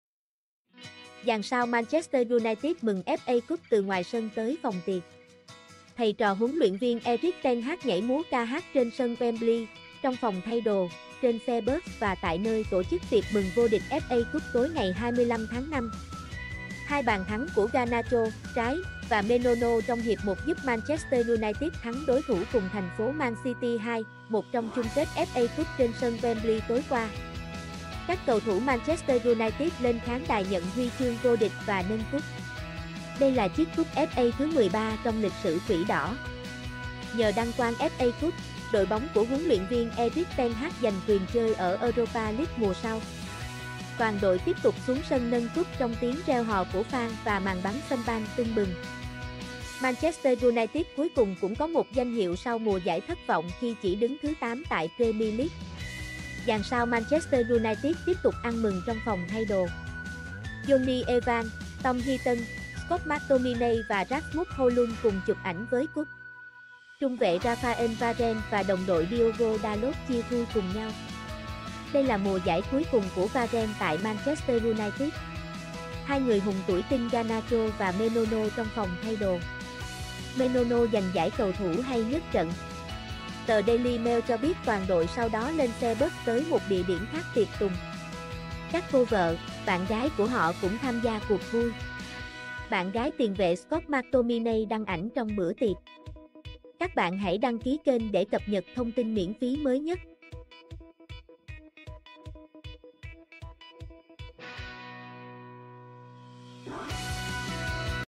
Dàn sao MU mừng FA Cup từ ngoài sân tới phòng tiệc
Thầy trò HLV Erik ten Hag nhảy múa ca hát trên sân Wembley, trong phòng thay đồ, trên xe bus và tại nơi tổ chức tiệc mừng vô địch FA Cup tối 25/5.

Hai bàn thắng của Garnacho (trái) và Mainoo trong hiệp một giúp MU thắng đối thủ cùng thành phố Man City 2-1 trong chung kết FA Cup trên sân Wembley tối qua.

Các cầu thủ MU lên khán đài nhận huy chương vô địch và nâng Cup. Đây là chiếc Cup FA thứ 13 trong lịch sử Quỷ đỏ. Nhờ đăng quang FA Cup, đội bóng của HLV Erik ten Hag giành quyền chơi ở Europa League mùa sau.

Toàn đội tiếp tục xuống sân nâng Cup trong tiếng reo hò của fan và màn bắn champagne tưng bừng. MU cuối cùng cũng có một danh hiệu sau mùa giải thất vọng khi chỉ đứng thứ 8 tại Premiere League.

Dàn sao MU tiếp tục ăn mừng trong phòng thay đồ. Jonny Evans, Tom Heaton, Scott McTominay và Rasmus Hojlund cùng chụp ảnh với Cup.

Trung vệ Raphael Varane và đồng đội Diogo Dalot chia vui cùng nhau. Đây là mùa giải cuối cùng của Varane tại MU.

Hai người hùng tuổi teen Garnacho và Mainoo trong phòng thay đồ. Mainoo giành giải Cầu thủ hay nhất trận.

Tờ Daily Mail cho biết toàn đội sau đó lên xe bus tới một địa điểm khác tiệc tùng. Các cô vợ, bạn gái của họ cũng tham gia cuộc vui. Bạn gái tiền vệ Scott McTominay đăng ảnh trong bữa tiệc.